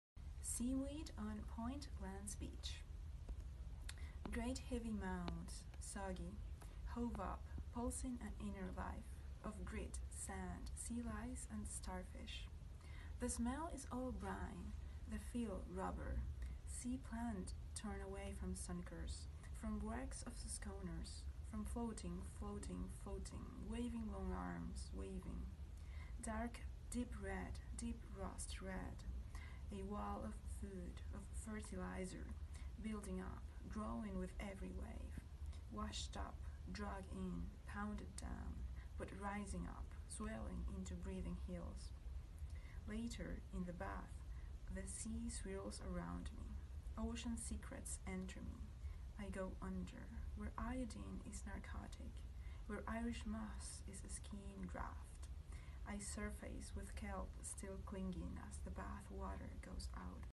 AtentadoPoético2019 ✈
Colaboración de @obnubilada leyendo Agnes Walsh, Seaweed on Point Lance Beach